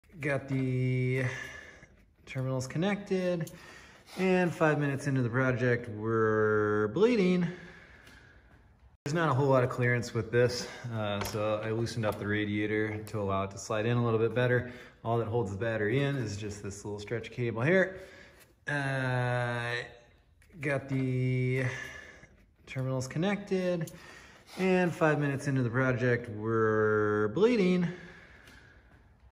How to Connect Terminals for a Radiator Project: Step-by-Step Guide
In this video, we demonstrate the process of connecting terminals for a radiator project. Learn how to ensure a secure connection and avoid any unexpected bleeding during the project.